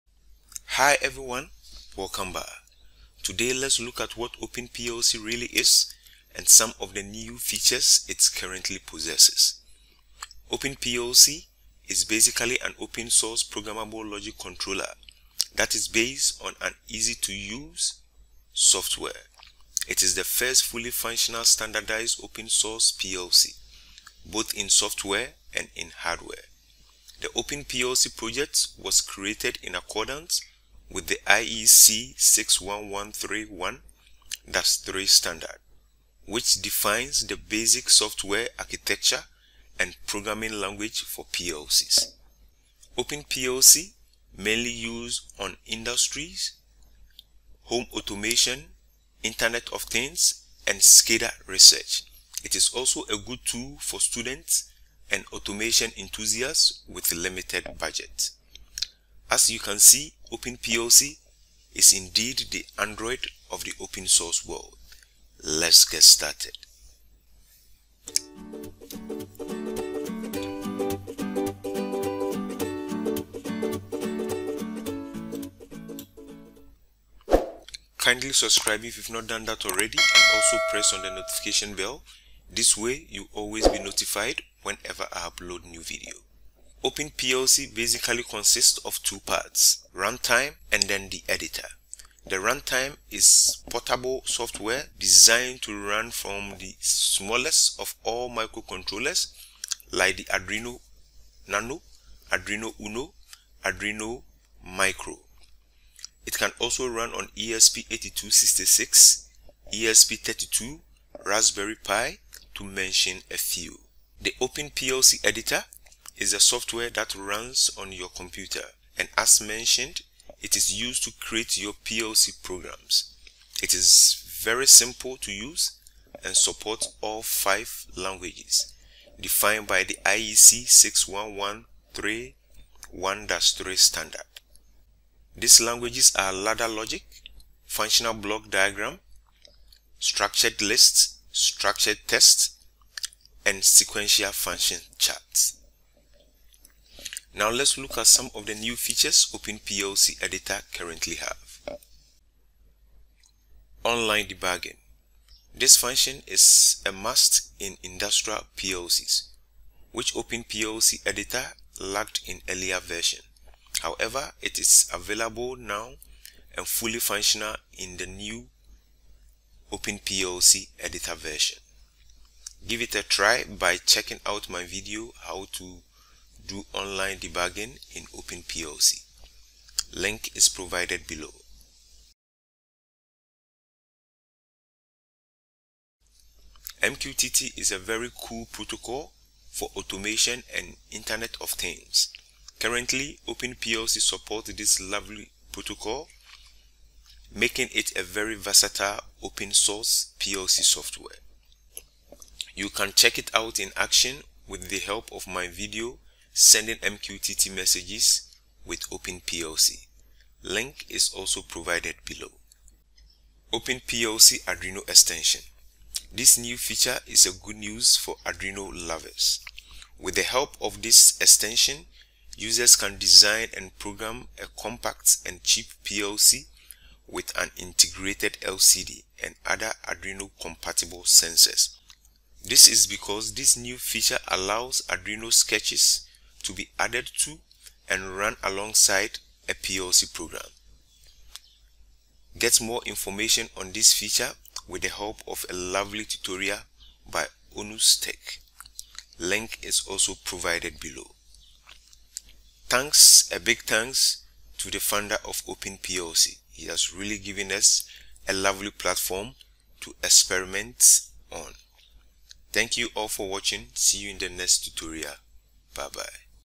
OpenPLC what's New
OpenPLC is an open-source Programmable Logic Controller that is based on an easy-to-use software. It is the first fully functional standardized open-source PLC, both in software and in hardware.